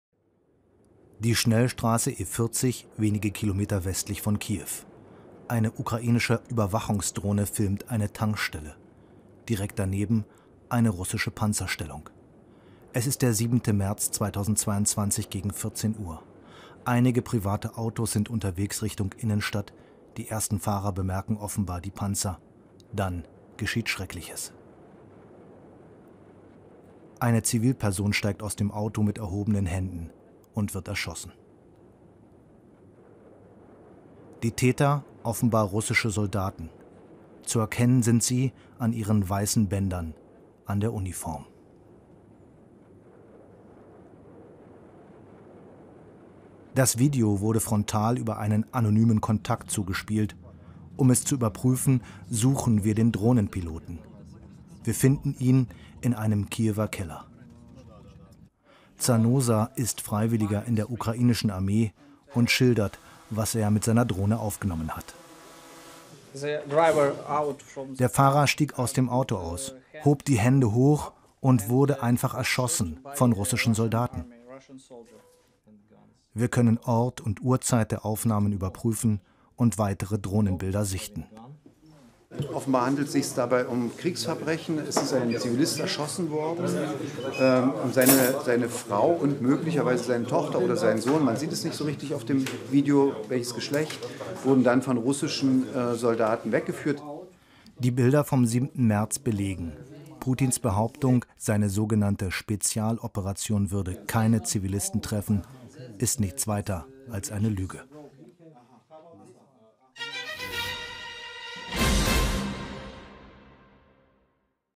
Another video of the killing of a civilian by pigs near KievUnarmed
arms raised.  Russian occupiers kill civilians near Kiev on March 7.  A video that the Ukrainian military passed on to German journalists